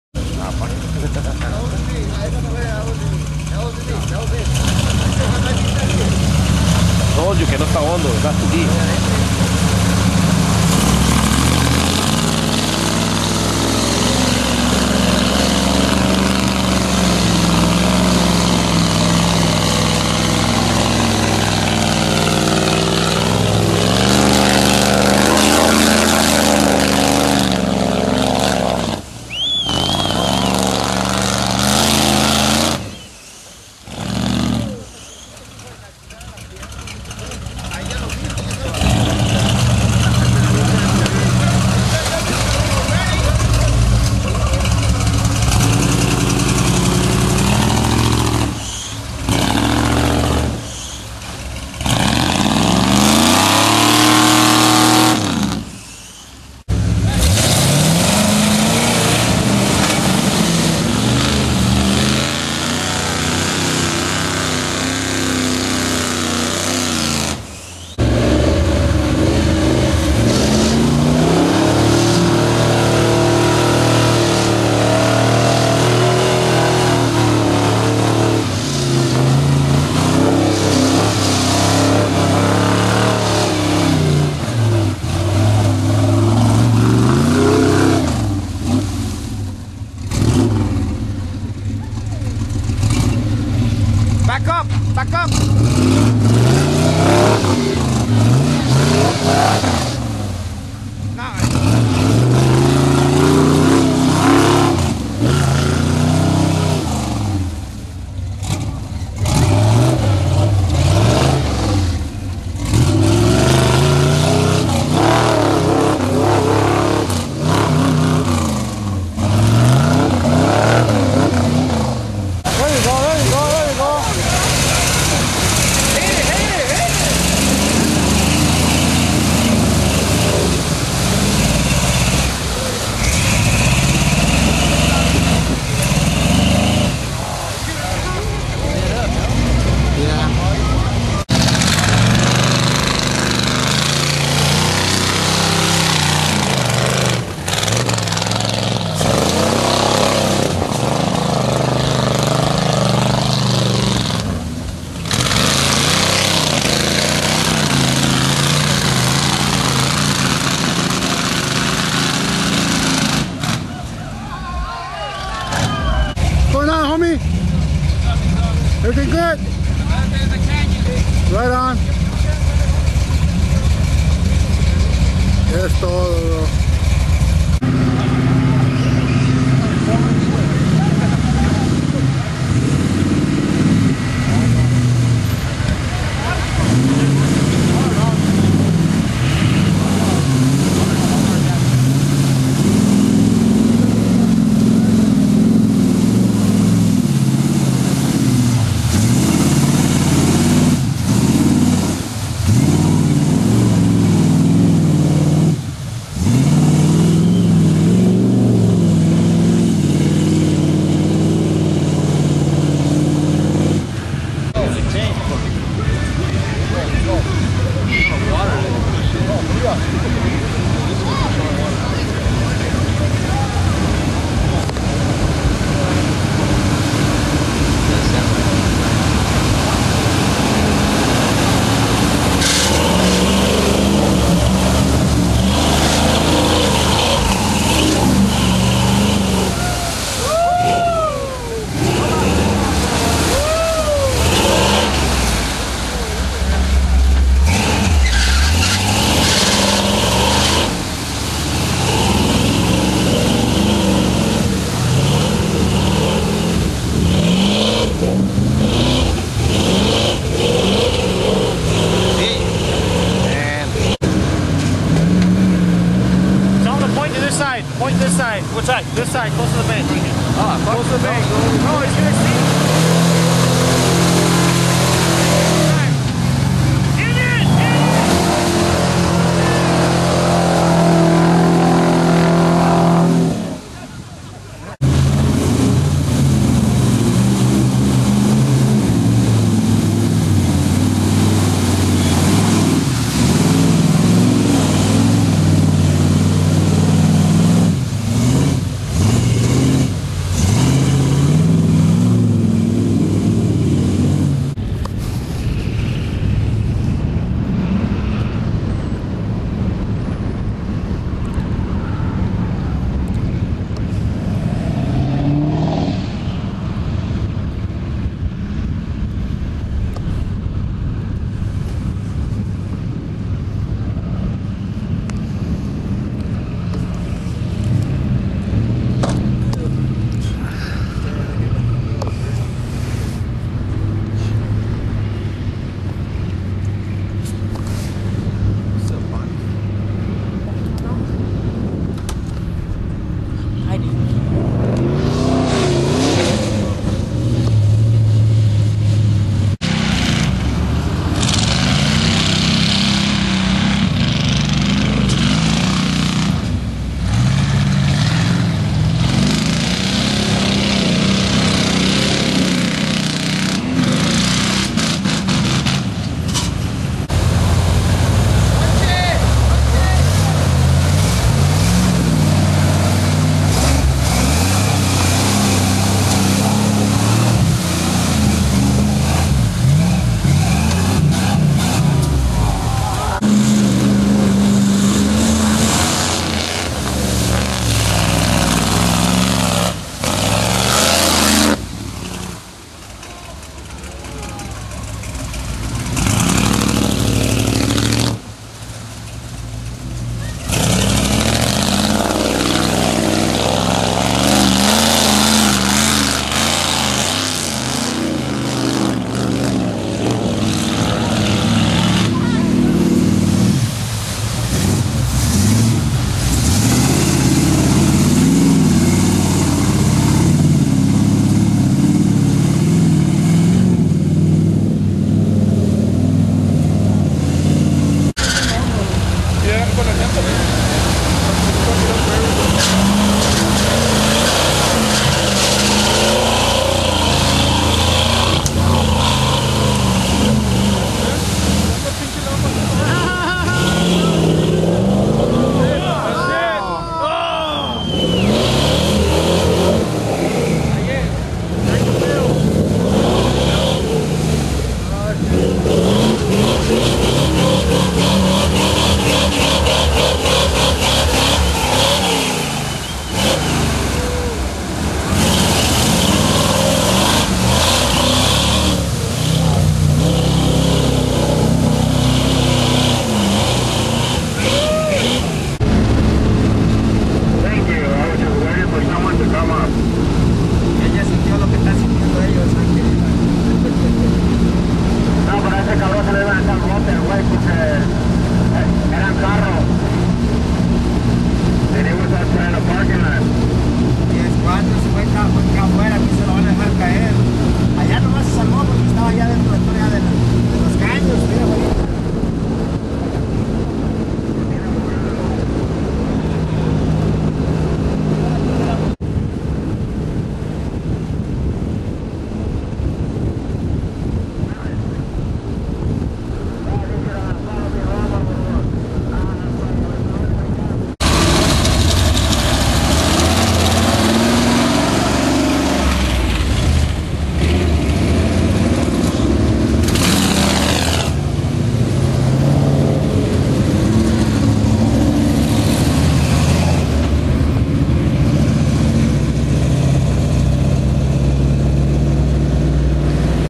K5s mudding PIGS SOLANOS OFF ROAD 4/15/12
it had rained during the week. the canyon got some more water